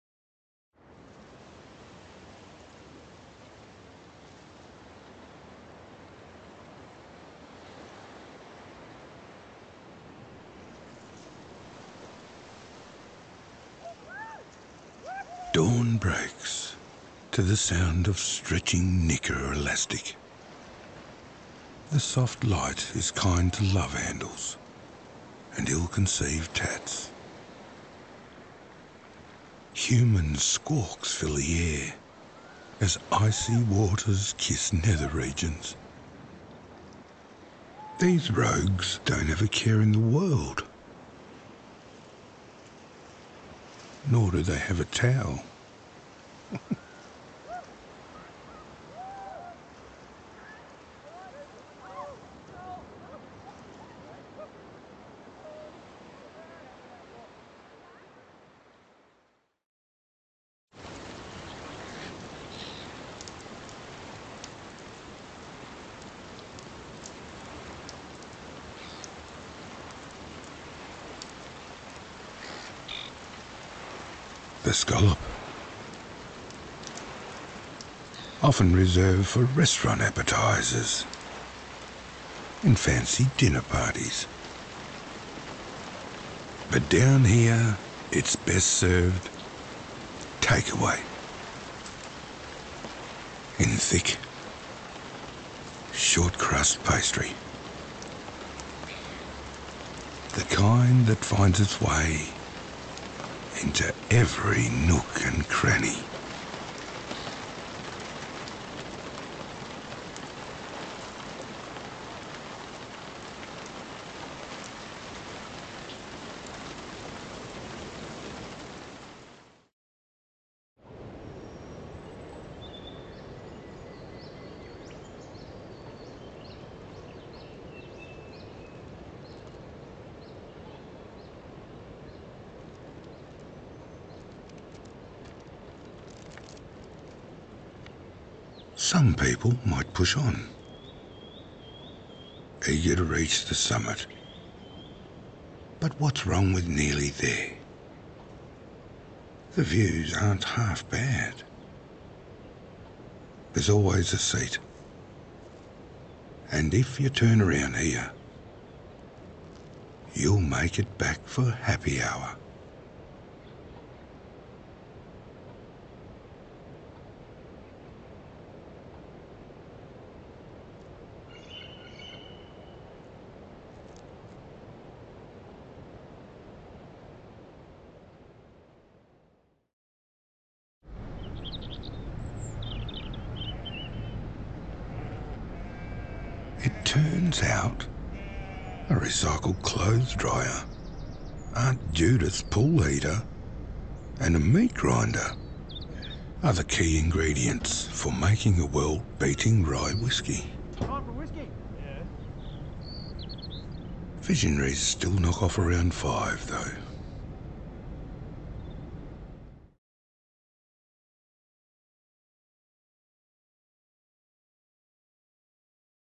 Tourism Tasmania: Come Down for Air
A beautifully paced tourism ad. The space in the words reflects the space surrounding the tourists. Magical.